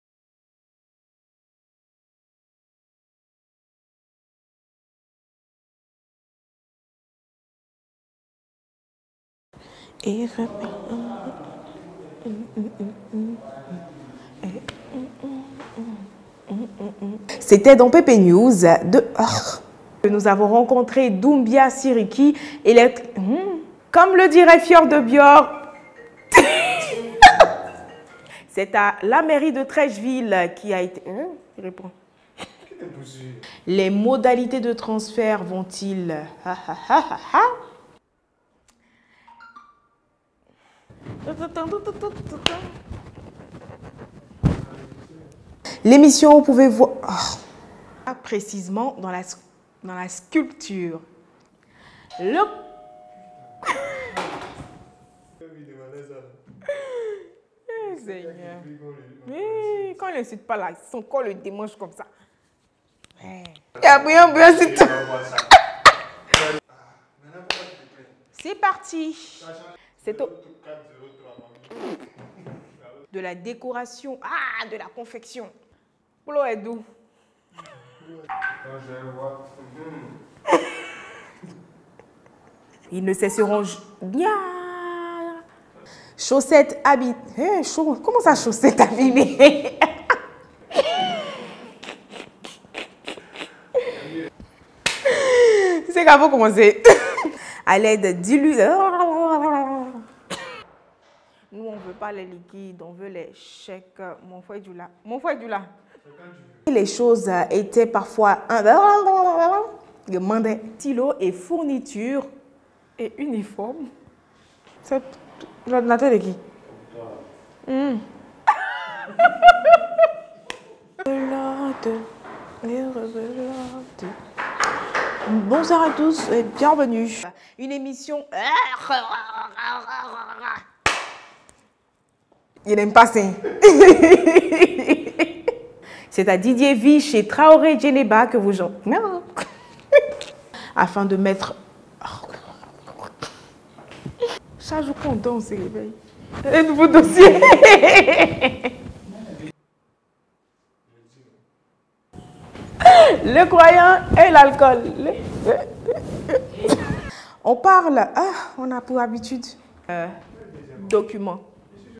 Les bêtisiers de Yuna Shimizu
🤦🏾‍♀️🤷🏽‍♀️🙋🏾‍♂️Découvrez votre animatrice dans des situations inattendues 😂Les bêtisiers de Yuna Shimizu 😁🎬 Restez connectés, sur PepeSoupe pour ne rien manquer de l'actualité ivoirienne.